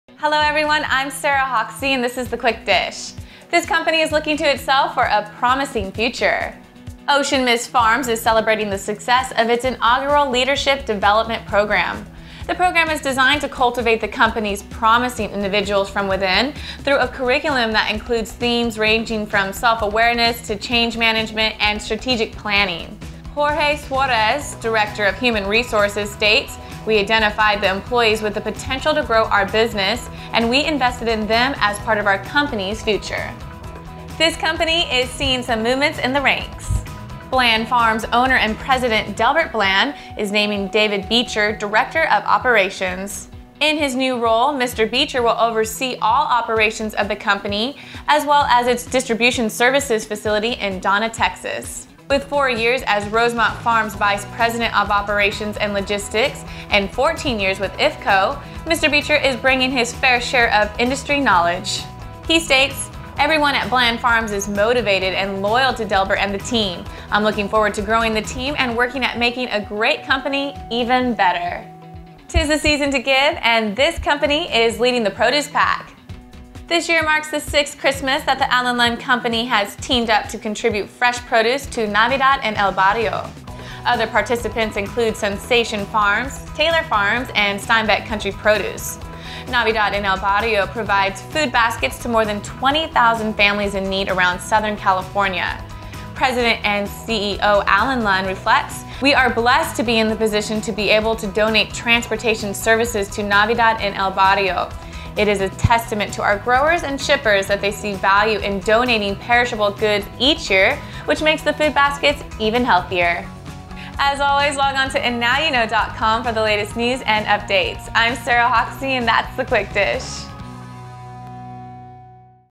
AndNowUKnow: Ocean Mist, Bland Farms, Allen Lund - Quick Dish
Ocean Mist Farms is celebrating the success of its inaugural leadership development program, graduating its first class in a ceremony on Nov. 19.  The program is designed to cultivate the company's promising individuals from within, through a curriculum that includes themes ranging from self-awareness to change management and strategic planning.  Jorge Suarez, Director of Human Resources for Ocean Mist, states, "This program is about developing key human capital at Ocean Mist Farms."  Mr. Suarez continues, "We identified the employees with the potential to grow our business, and we invested in them as part of our company's future."

   Bland Farms Owner and President, Delbert Bland, is naming David Beecher, Director of Operations. In his new role Mr. Beecher will oversee all operations of the company as well as its Distribution Services facility in Donna, Texas.  With four years as Rosemont Farms' Vice President of Operations and Logistics and fourteen years with IFCO, Mr. Beecher is bringing his fair share of industry knowledge.  The new Director of Operations states, "Everyone at Bland Farms is motivated and loyal to Delbert and the team. I'm looking forward to growing the team and working at making a great company even better."

   This year marks the sixth Christmas that the Allen Lund Company has teamed up to contribute fresh produce to Navidad en el Barrio. Also participating are Salinas, Central Valley and Yuma, AZ growers that include Sunsation Farms, Inc. from Monterey, CA, Fillmore Piru Citrus of Fillmore, CA, as well as Taylor Farms and Steinbeck Country Produce of Salinas, CA. The non-profit organization, Navidad en el Barrio, provides food baskets to more than 20,000 families at or near the poverty level around Southern California at Christmas. The Allen Lund Company will be donating the cost of transporting the produce. Allen Lund, President and CEO of the company reflects, "We are blessed to be in the position to be able to donate transportation services to Navidad en el Barrio...it is a testament to our growers and shippers that they see value in donating perishable goods each year, which makes the food baskets even healthier."